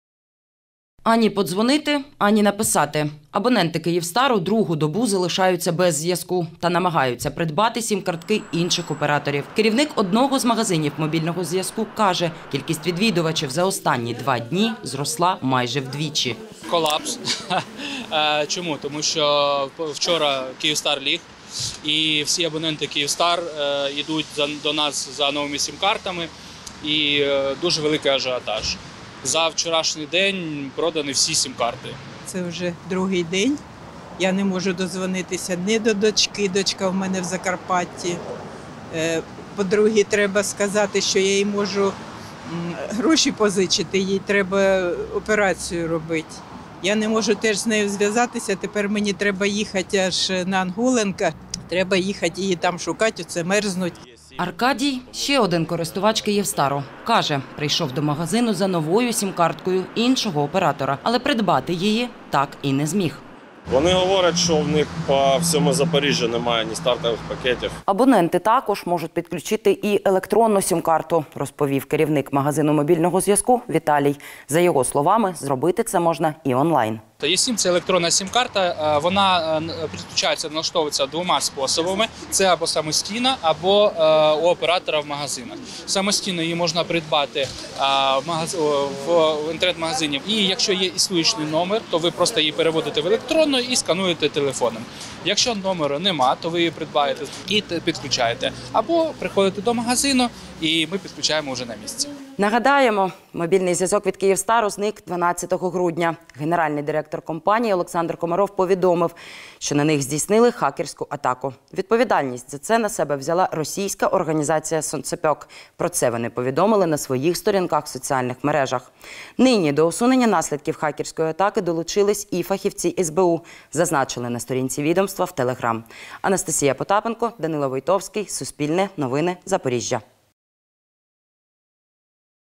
Через збій «Київстар» у Запоріжжі люди скуповують сім-карти інших операторів
Через хакерську атаку в Запоріжжі вже другу добу немає зв'язку оператора «Київстар». Зранку, 13 грудня, люди вишикувались в черги аби купити сімку інших операторів. Наразі в офіційних пунктах продажу стартових пакетів вже майже немає, а в Інтернеті їх продають втричі дорожче.